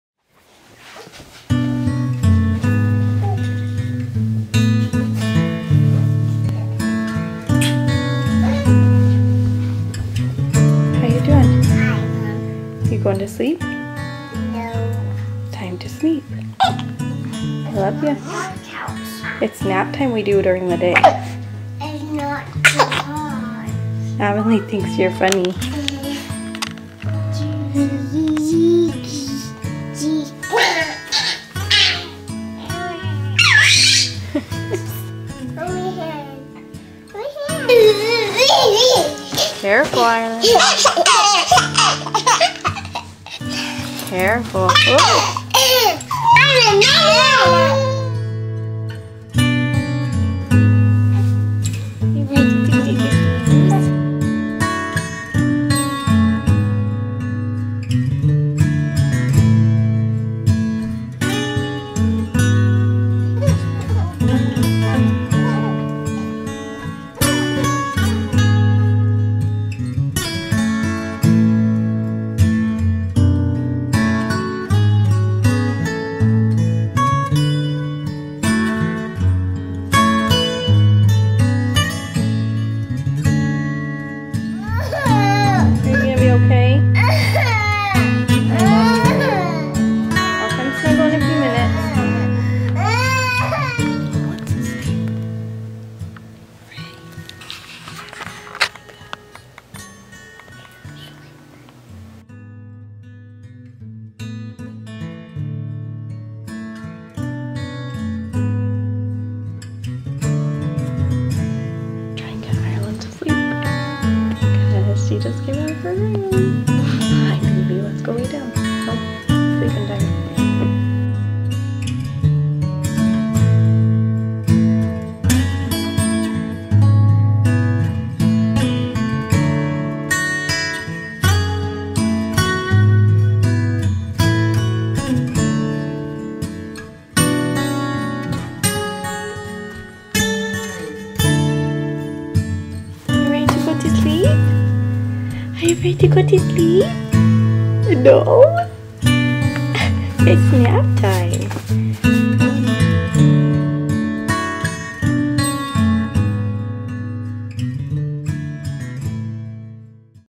Nap Time Diaries
A typical nap time at the Ashby house. Sometimes I do a "Nap Time Diaries" on my snapchat (emmafashby) so this is just a spin off of that!